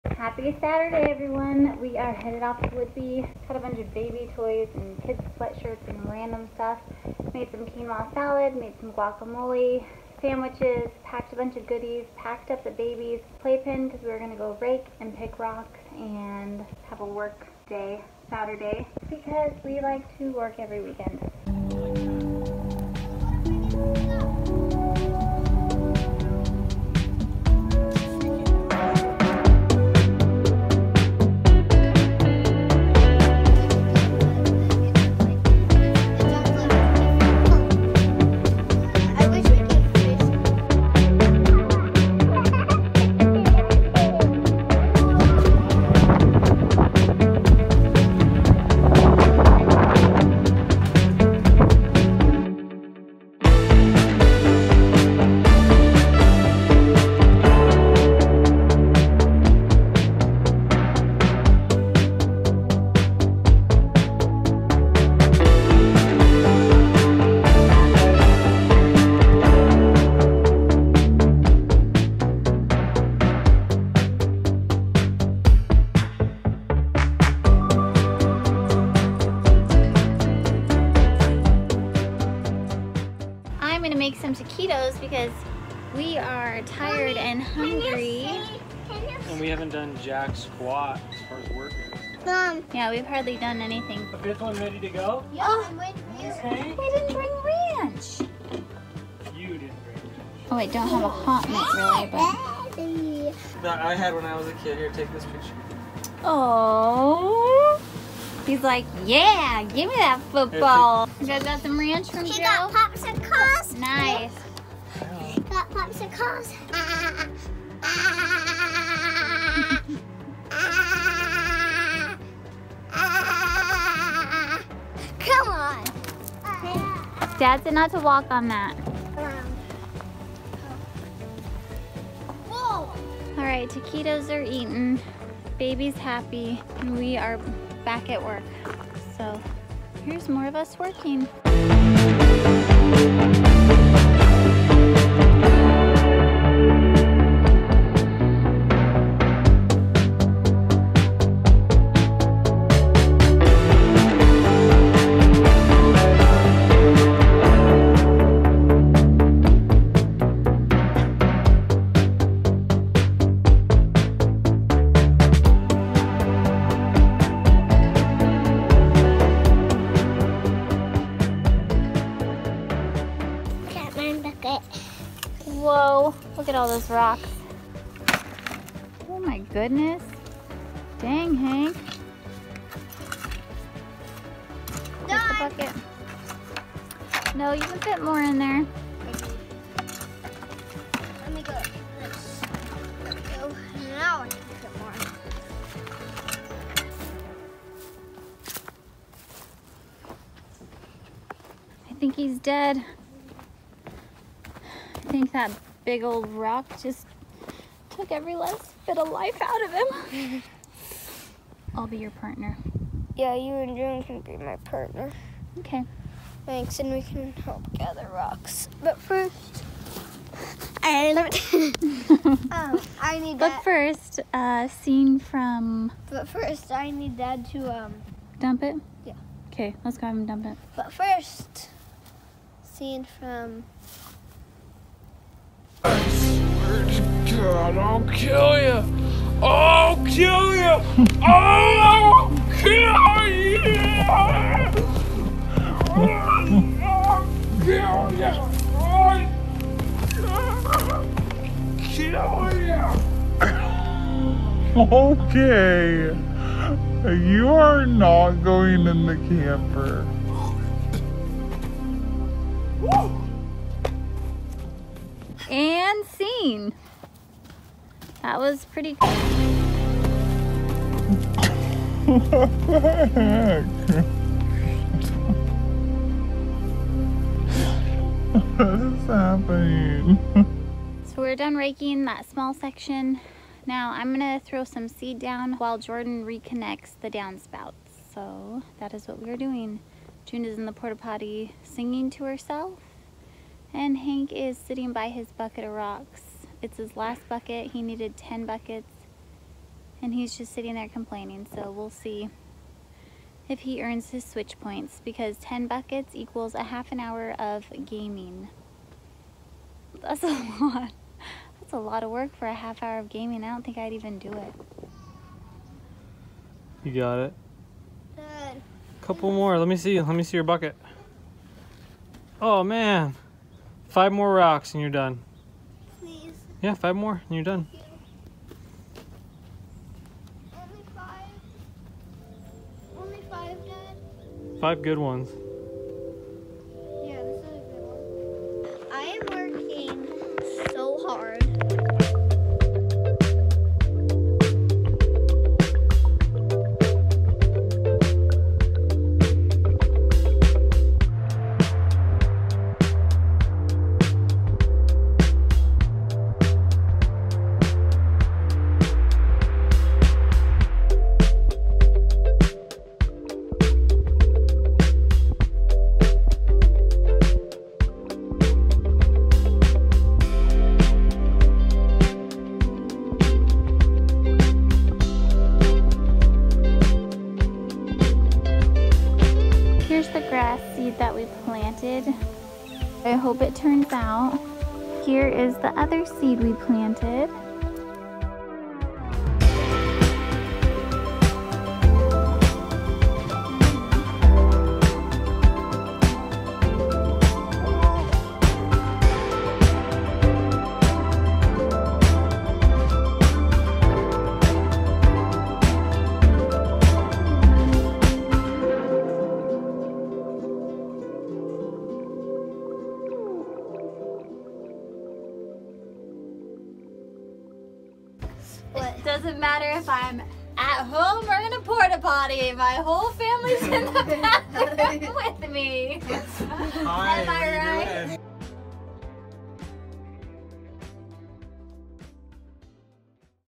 Work Hard, Play Hard
Family work day out at the cabin, with an epic cinematic feature around half way through :)